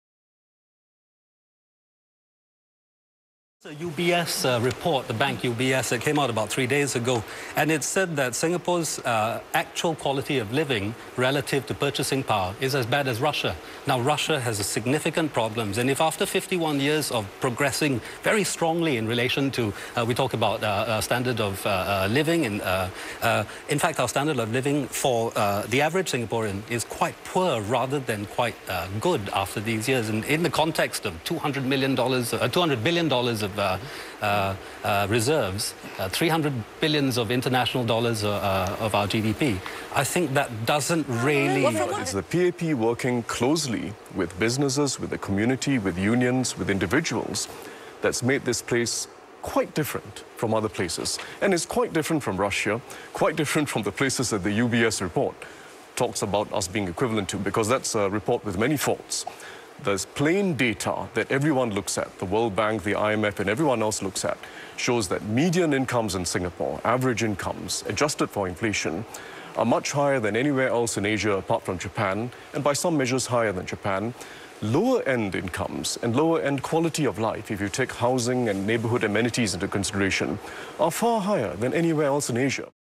Singapore GE 2011 - Standard of Living
Debate on CNA between PAP's Shanmugaratnam and SDP's Wijeysingha on standard of living (the UBS report)